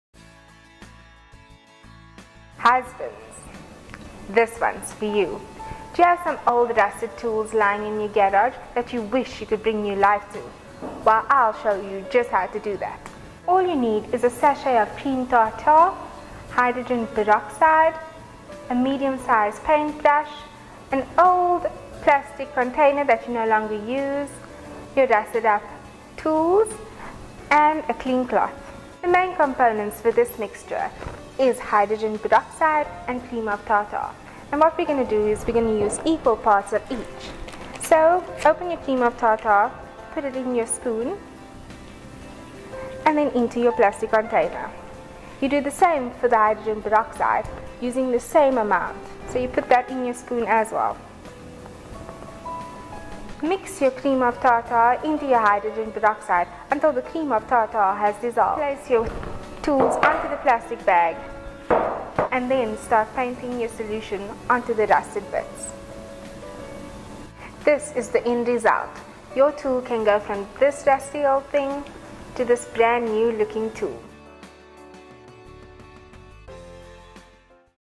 TIP: How to remove rust from tools
Things happen - and tools at times get really badly rusted. Here's a quick and easy way to give those rusty bits a new leash on life.